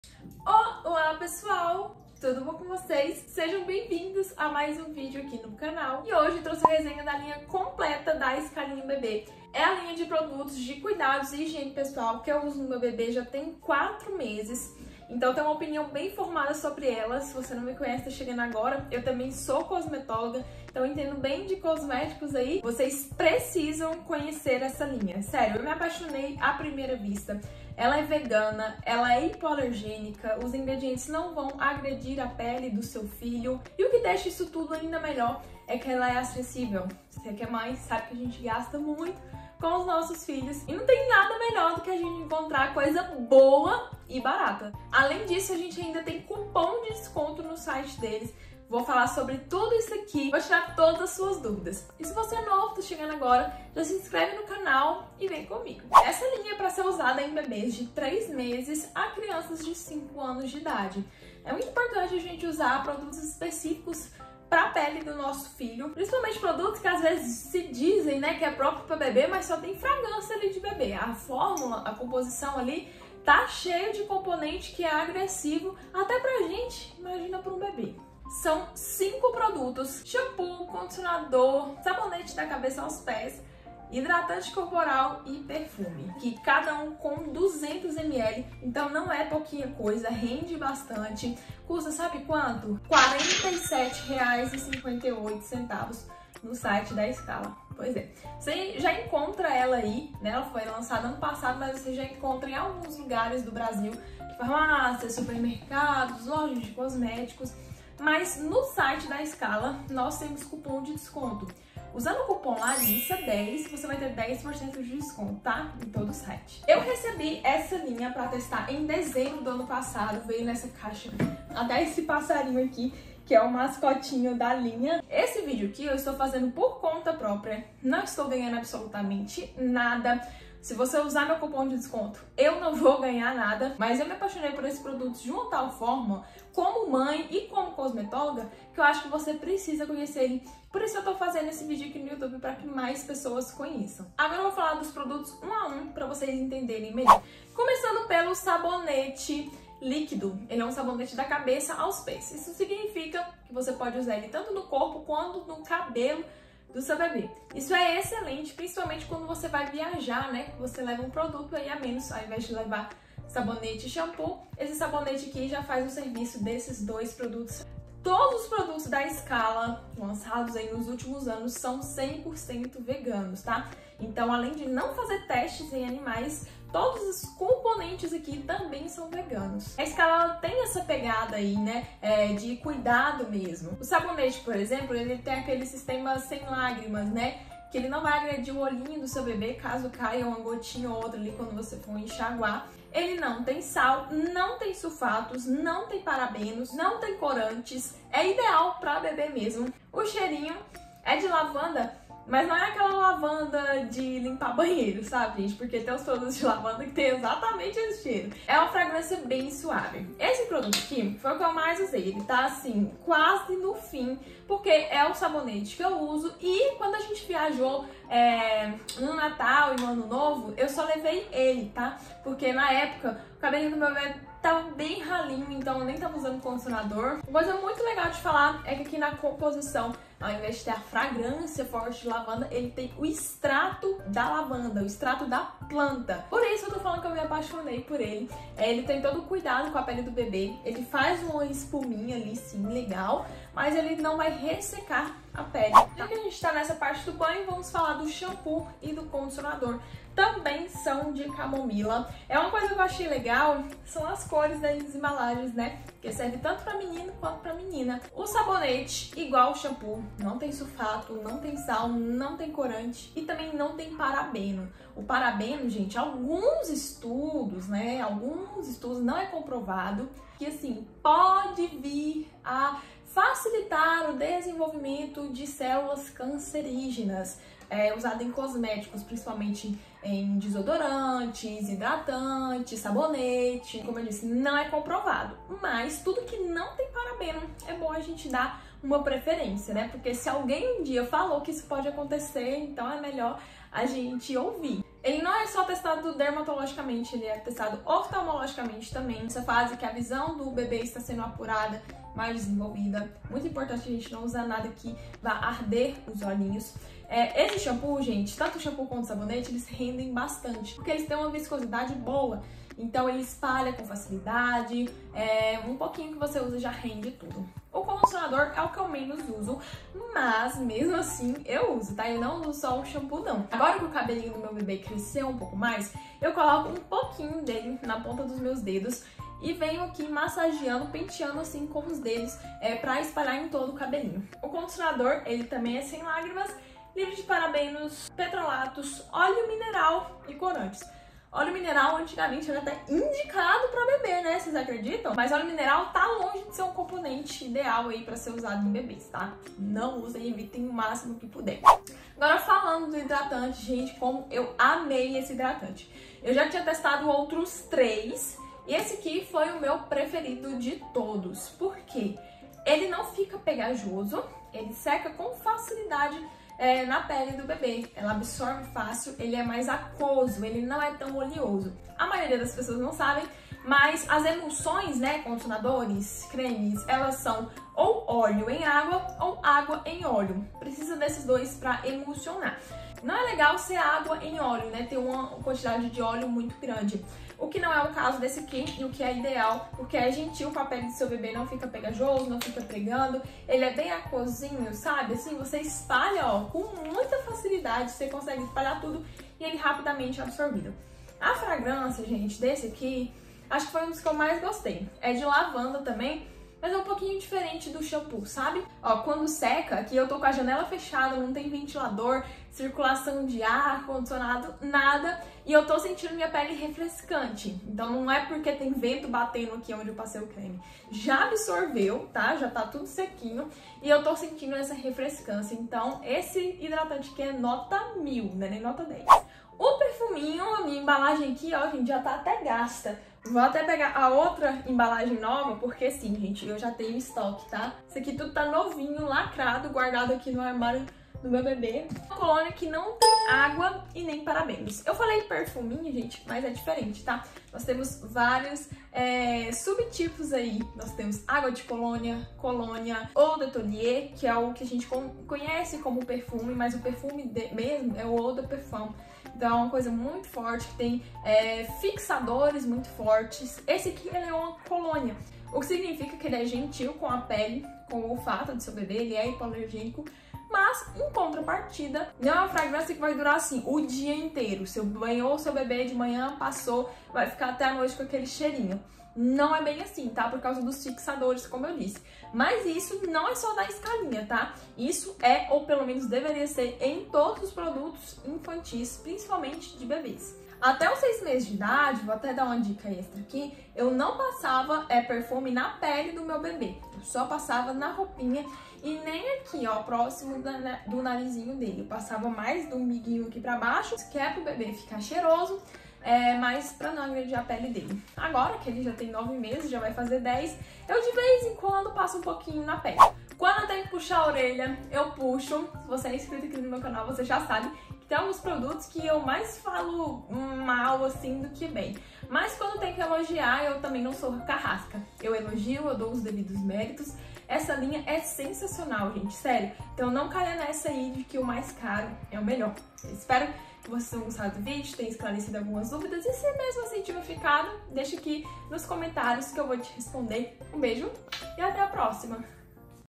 SKALINHA BEBÊ - Resenha completa da linha
Depois de 4 meses usando a linha Skalinha Bebê, trouxe a resenha completa para vocês, explicando os benefícios e se realmente vale a pena adquirir essa linha Skalinha Bebê.

Espero que vocês gostem do Skalinha Bebê assim como eu, tenho certeza que vai fazer sucesso com seu bebê também!